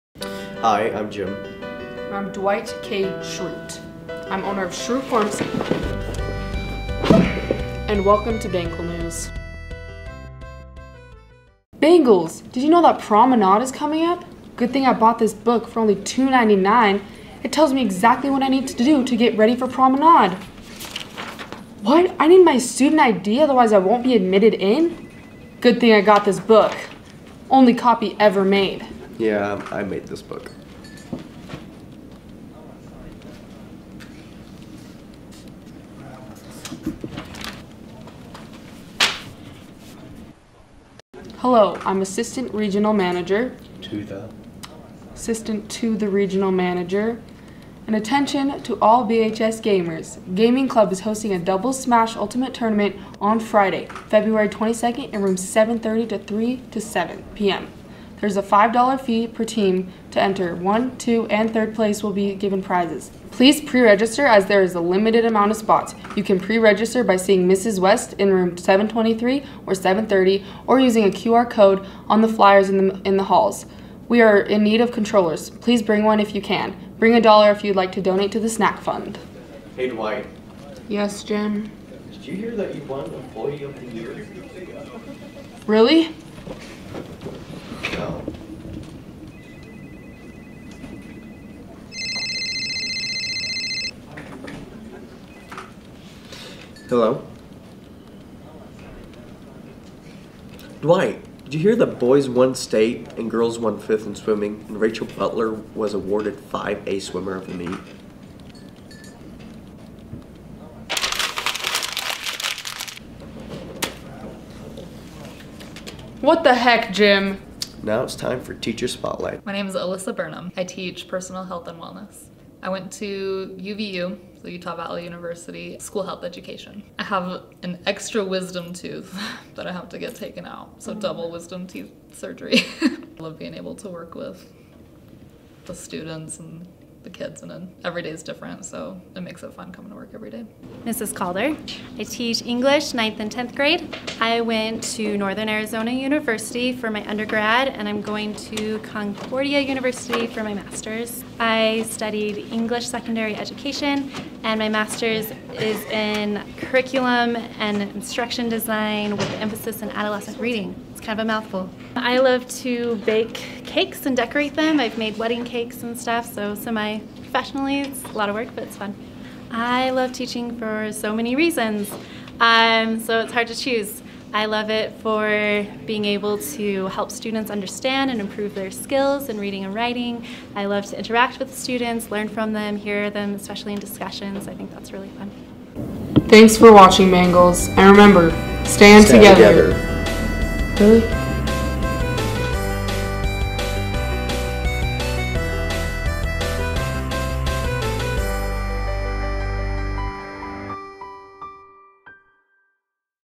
Bengal News Office Parody
High School Newscast Nominee
10th Anniversary Canyons District Film Festival
by Zoe Berg - Brighton High School